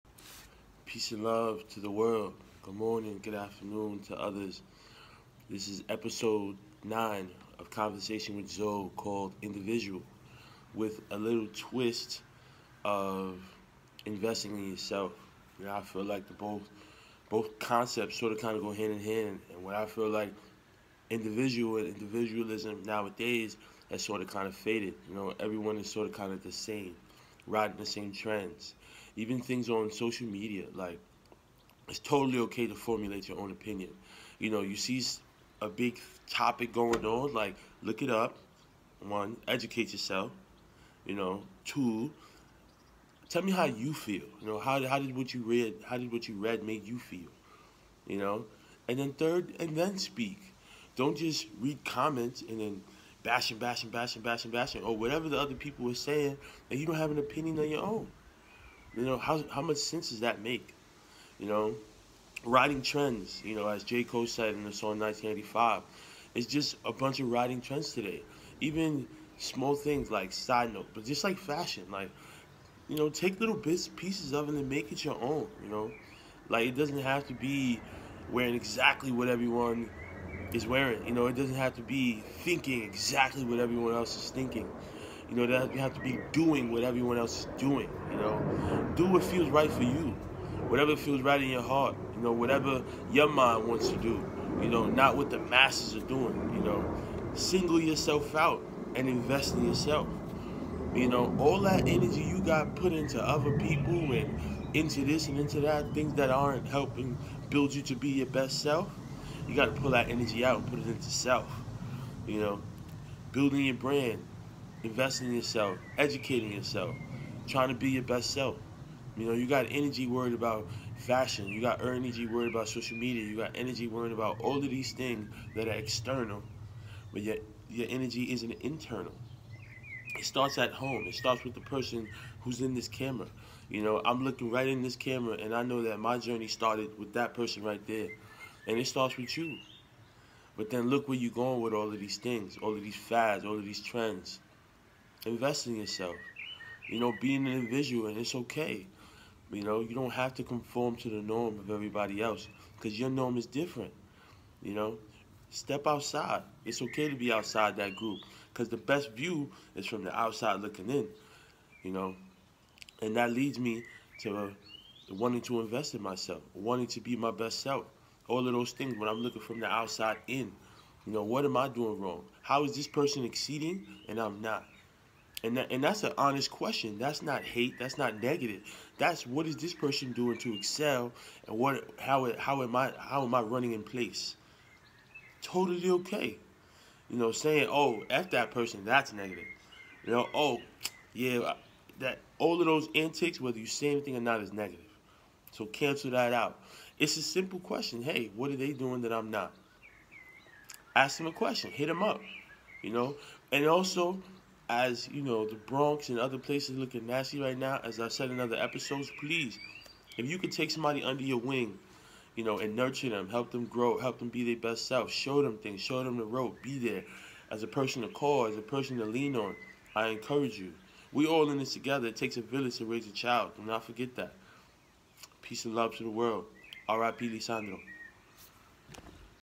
Conversation With Zo Episode 9 “Individual/Invest In Yourself” Create and Bring your own value
Be you, be yourself, form your own opinions. Start your own trend and be comfortable walking in your own shoes.